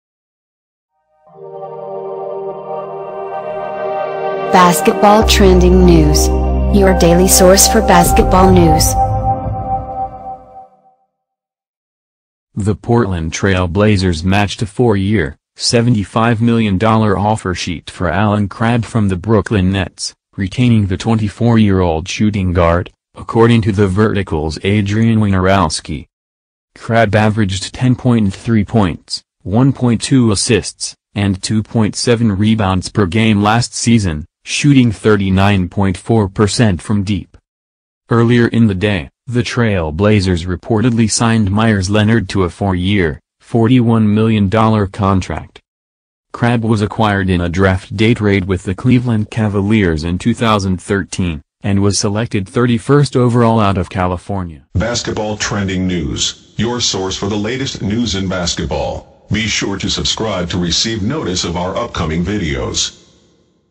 Blazers match Allen Crabbe's 4-year, $75M offer sheet from Nets
Blazers match Crabbe's 4-year, $75M offer sheet from Nets